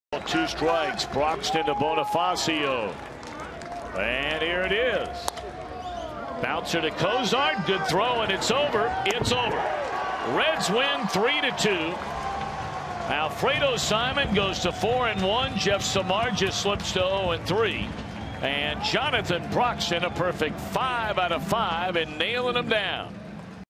CHC@CIN: Broxton retires Bonifacio for the save
4/29/14: Jonathan Broxton induces a groundout to complete a 1-2-3 9th for the save